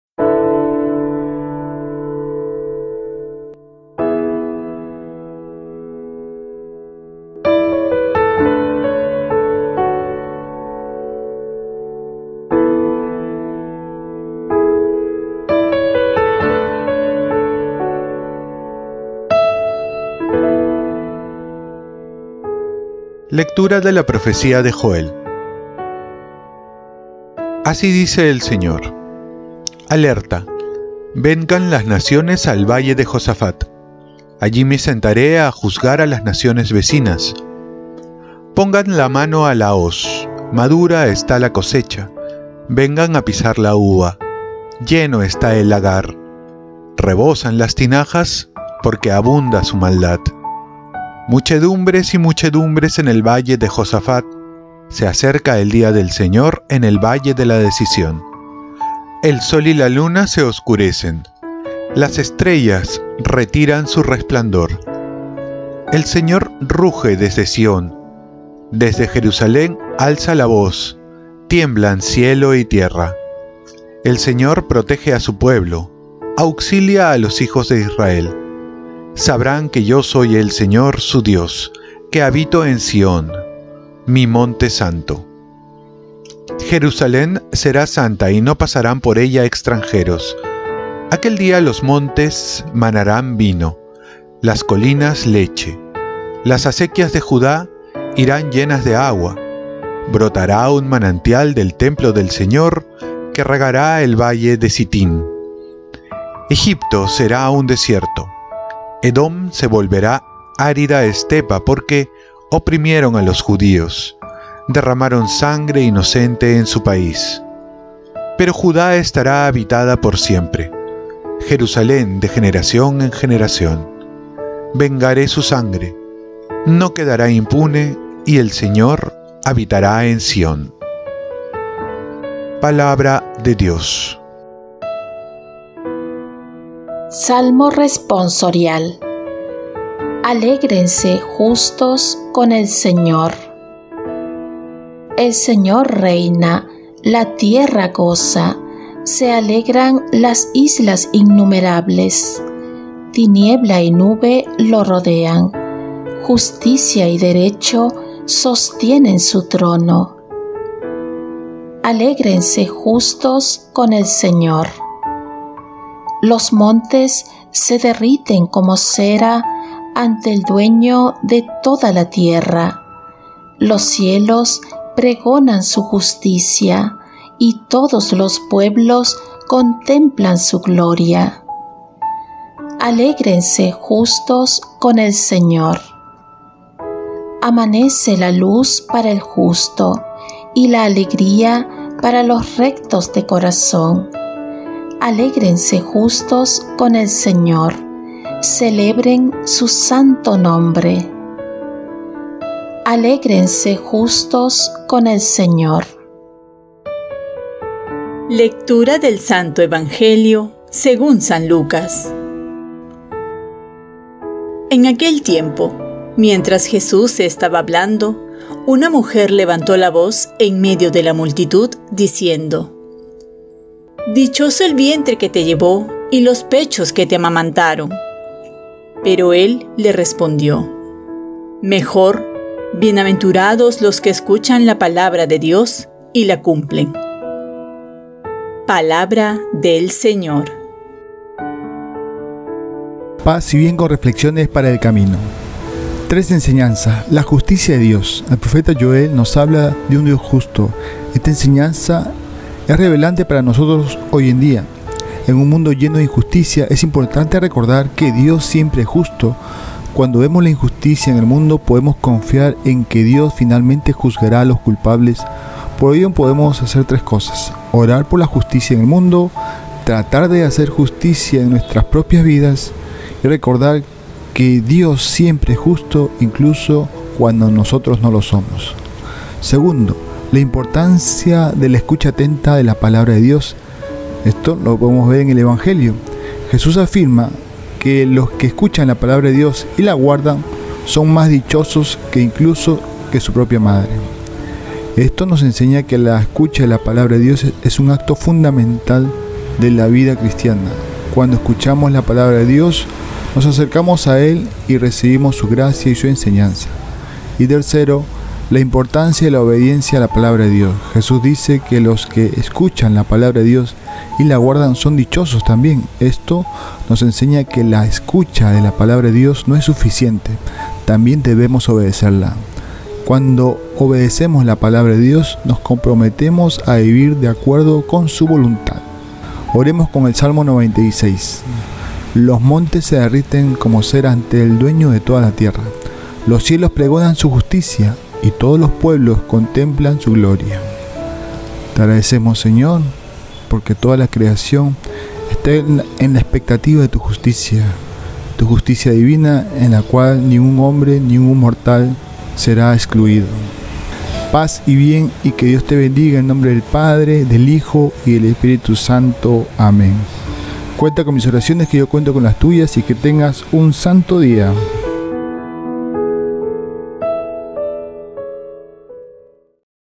Evangelio del sábado 14 de octubre 2023-XXVII Semana Tiempo Ordinario-Hno David Pacheco OFM Cap
📖 Lectura de la profecía de Joel 4, 12-21
Así dice el Señor: «Alerta, vengan las naciones al valle de Josafat: allí me sentaré a juzgar a las naciones vecinas. Pongan mano a la hoz, madura está la cosecha; vengan a pisar la uva, lleno está el lagar. Rebosan las tinajas, porque abunda su maldad. Muchedumbres y muchedumbres en el valle de Josafat, se acerca el día del Señor en el valle de la Decisión. El sol y la luna se oscurecen, las estrellas retiran su resplandor. El Señor ruge desde Sion, desde Jerusalén alza la voz, tiemblan cielo y tierra. El Señor protege a su pueblo, auxilia a los hijos de Israel. Sabrán que yo soy el Señor, su Dios, que habito en Sion, mi monte santo. Jerusalén será santa, y no pasarán por ella extranjeros. Aquel día, los montes manarán vino, las colinas, leche; las acequias de Judá irán llenas de agua, brotará un manantial del templo del Señor, que regará el valle de Sitín. Egipto será un desierto, Edom se volverá árida estepa, porque oprimieron a los judíos, derramaron sangre inocente en su país. Pero Judá estará habitada por siempre, Jerusalén, de generación en generación. Vengaré su sangre, no quedará impune, y el Señor habitará en Sion».

En aquel tiempo, mientras Jesús estaba hablando, una mujer levantó la voz en medio de la multitud, diciendo: «Dichoso el vientre que te llevó y los pechos que te amamantaron». Pero Él le respondió: «Mejor, bienaventurados los que escuchan la Palabra de Dios y la cumplen».

😇 Reflexión
Tres enseñanzas
La justicia de Dios: El profeta Joel, nos habla de un Dios justo, esta enseñanza es relevante para nosotros hoy en día. En un mundo lleno de injusticia, es importante recordar que Dios siempre es justo. Cuando vemos la injusticia en el mundo, podemos confiar en que Dios finalmente juzgará a los culpables, por ello podemos hacer tres cosas: Orar por la justicia en el mundo, tratar de hacer justicia en nuestras propias vidas y recordar que Dios siempre es justo, incluso cuando nosotros no lo somos.
La importancia de la escucha atenta de la Palabra de Dios. Jesús afirma que los que escuchan la Palabra de Dios y la guardan son más dichosos que incluso su propia madre. Esto nos enseña que la escucha de la Palabra de Dios es un acto fundamental de la vida cristiana. Cuando escuchamos la Palabra de Dios, nos acercamos a Él y recibimos su gracia y su enseñanza.
La importancia de la obediencia a la Palabra de Dios. Jesús dice que los que escuchan la Palabra de Dios y la guardan son dichosos también. Esto nos enseña que la escucha de la Palabra de Dios no es suficiente, también debemos obedecerla. Cuando obedecemos la Palabra de Dios, nos comprometemos a vivir de acuerdo con su voluntad.

🙌 Oremos con el salmo 96
“Los montes se derriten como cera ante el dueño de toda la tierra; los cielos pregonan su justicia, y todos los pueblos contemplan su gloria”

Hno David Pacheco Neyra, OFM Cap